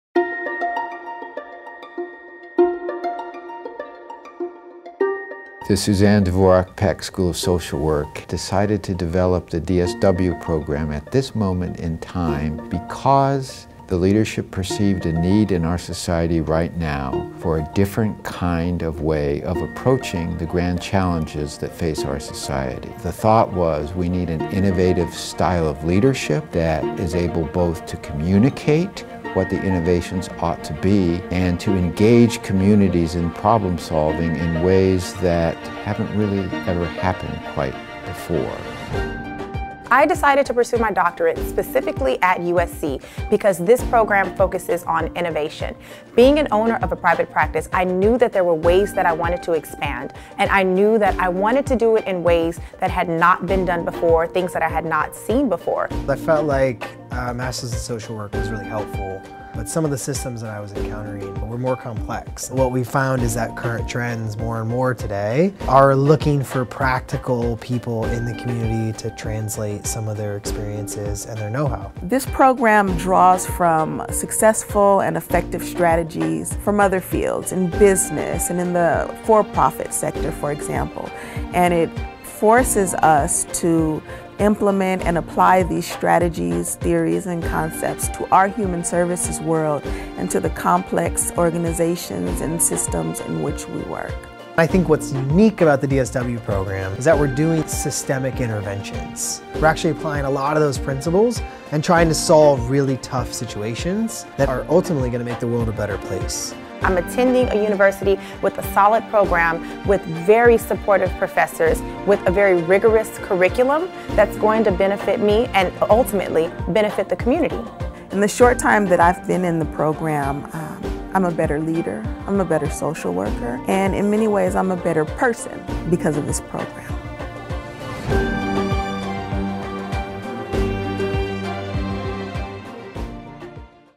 Doctor of Social Work (DSW) - USC Suzanne Dworak-Peck School of Social Work
The online DSW program at the USC Suzanne Dworak-Peck School of Social Work prepares experienced professionals seeking leadership or management positions to develop solutions to large-scale social problems.